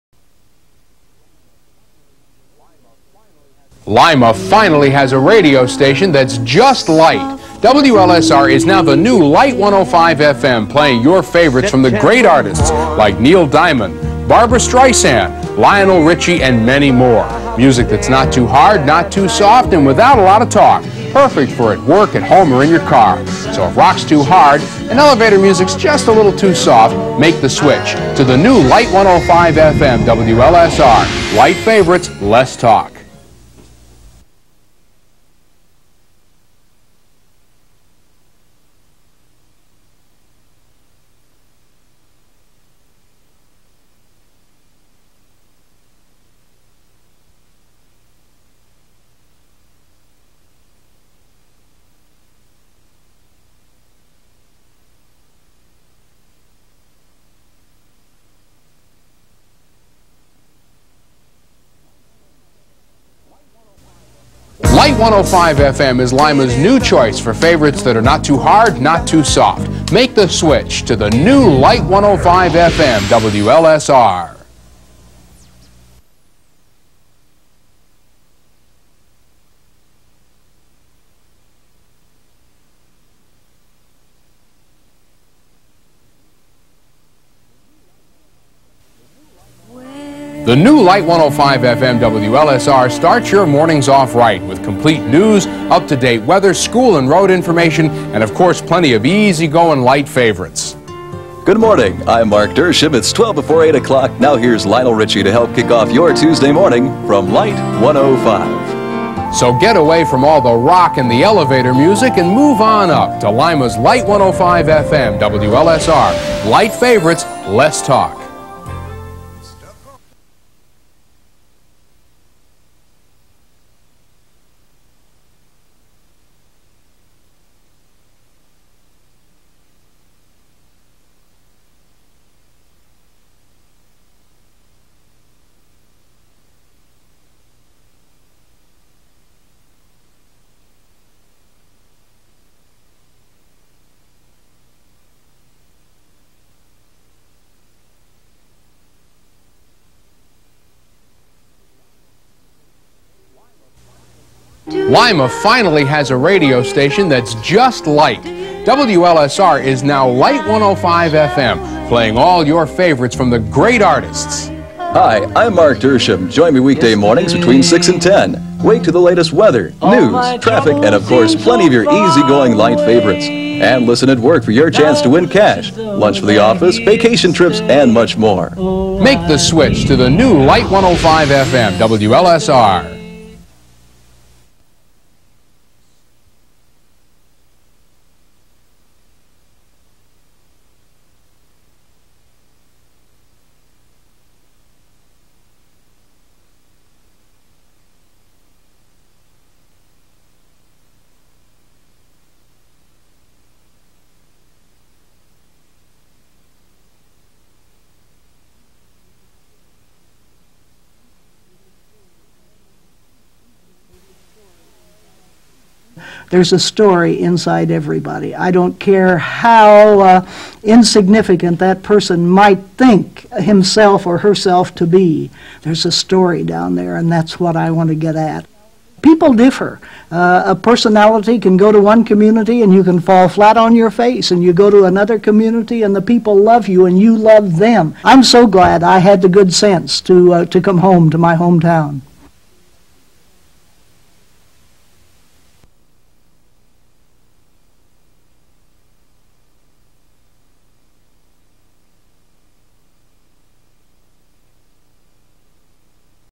WCIT:WLSR TV Spots
TV commercials promoting the new format for WLSR Radio, Lima, Ohio, rebranded as Lite 105 in 1986. Also a commercial featuring Legendary Lima broadcaster Easter Straker, who hosted a daily talk show on WCIT-AM from 1984 until 1993.